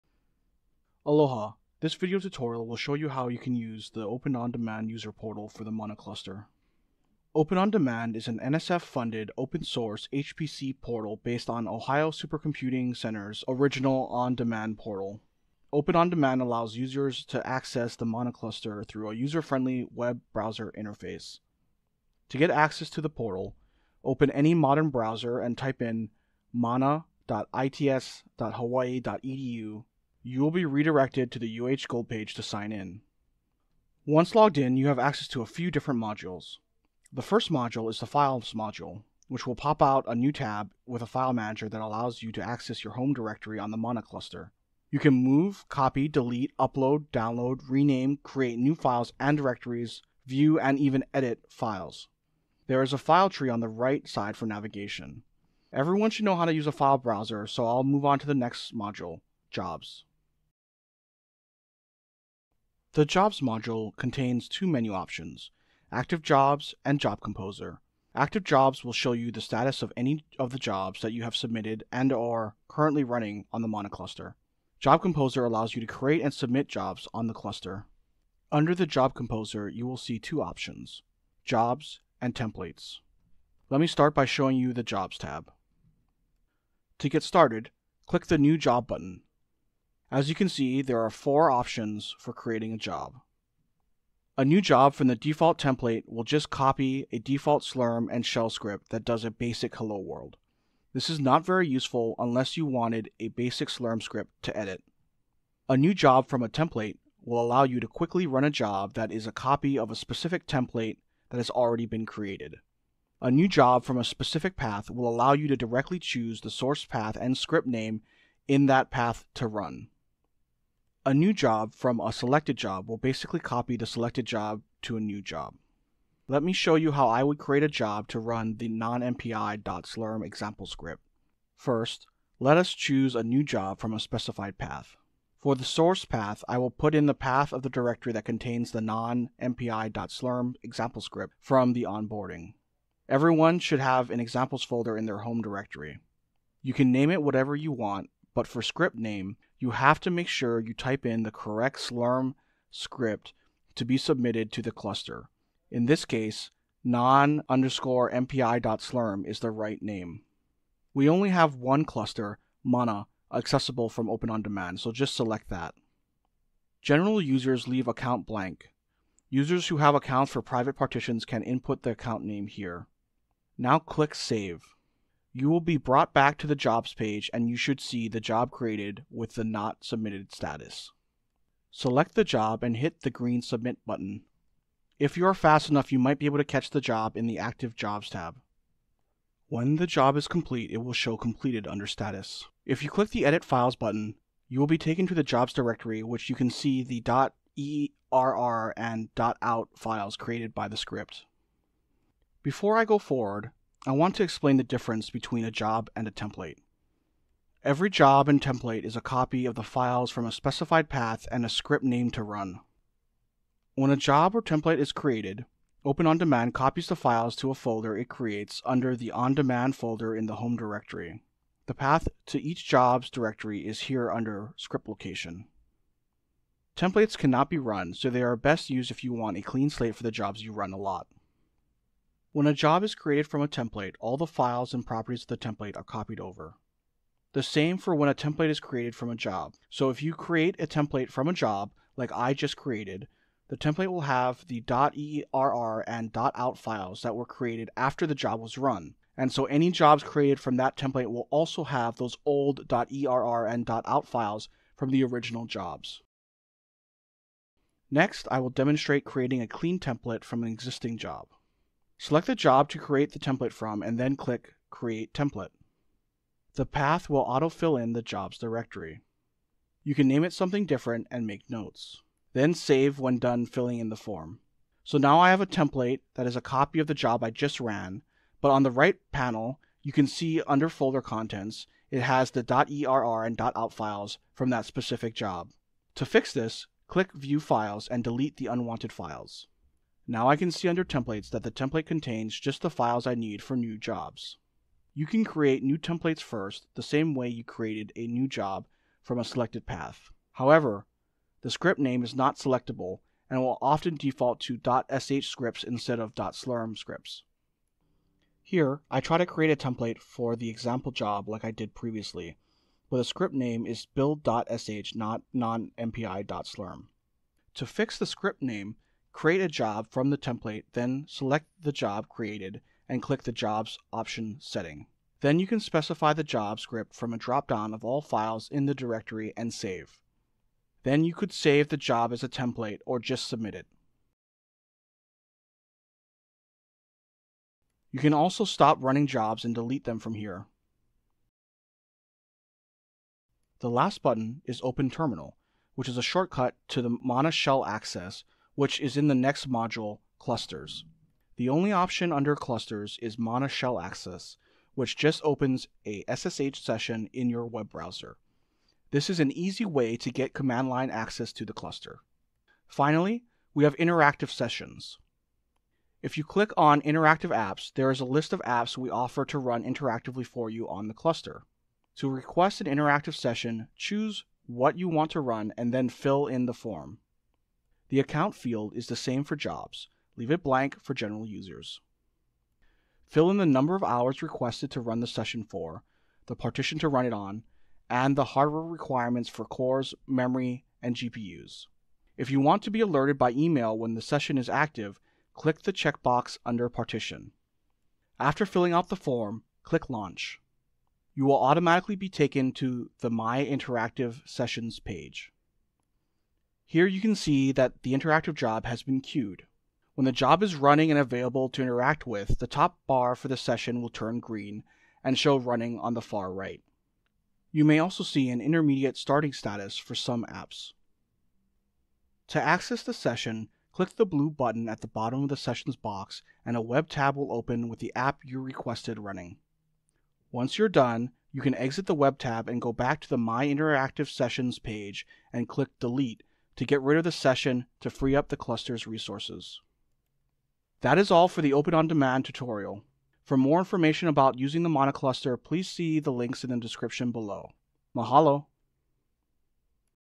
Using the Open OnDemand HPC Portal
Learn more about the National Science Foundation-supported open-source HPC portal, Open OnDemand. Learn how to access the UH Mana HPC using this user-friendly interface.